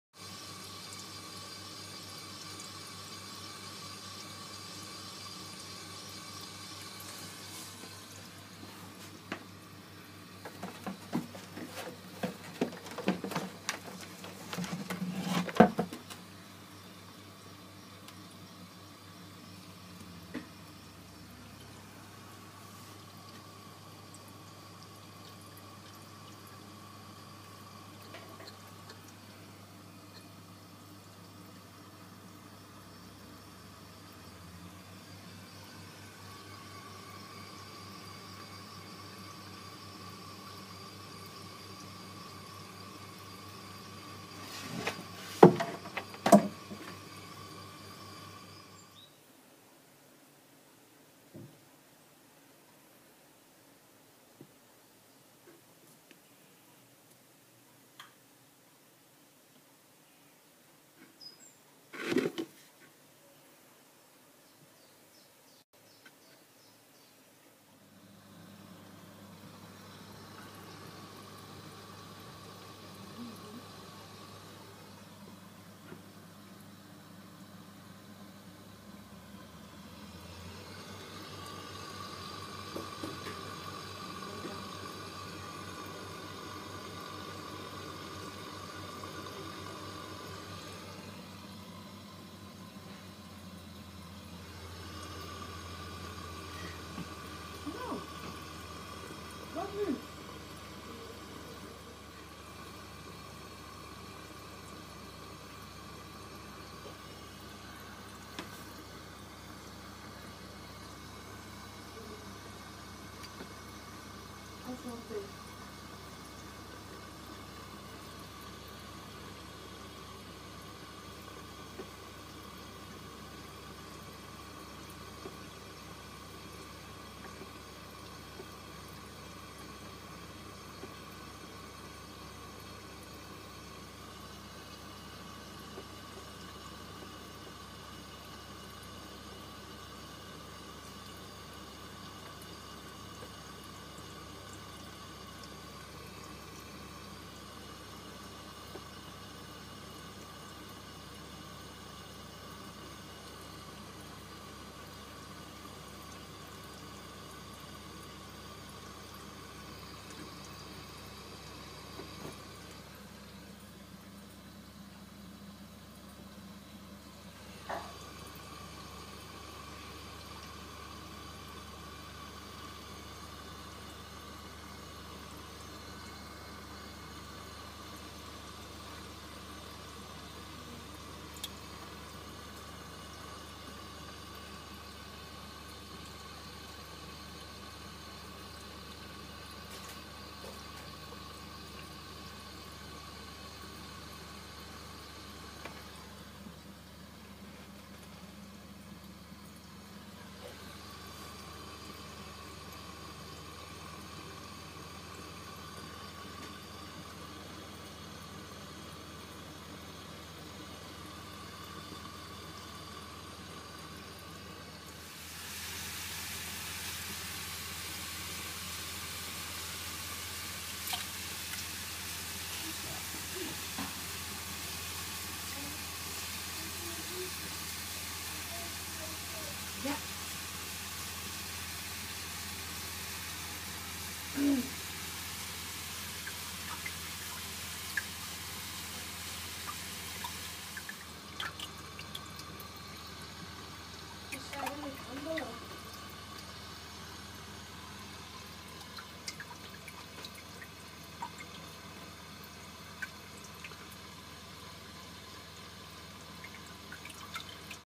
Adjusting Ati Pc 200i After Cleaning Venturi
About three weeks after the last cleaning of my skimmer pump, I noticed the air intake had dropped a lot. With a clean pump and venturi, the max air intake is 2500 l/h, but I was testing it the last two weeks at 1500 l/h. To do so, I had to dim down the pump to about 60%. But now the air intake at max pump power didn't exceed 1400 l/h.
So I opened the pump and noticed salt creep in the venturi. Cleaned it, and the max air intake was back at + 2000 l/h.
Then comes the trick of adjusting the skimmer again. Set the pump power so the air intake is 1500 l/h. Then raise the water level in the skimmerbody by closing the outlet. But this makes the air intake drop, so you have to turn up the pump power a little...but this makes the water level raise ...so, well as you see in the movie, it takes 5 minutes to set it up.
Notice that I just fed my fish an hour ago, and therefore the foam isn't as fine as it will be in a few hours. Then I have to make some adjustments again.

Wonder why ATI has just announced the IS version of these skimmers, with automated regulation of the airintake ;)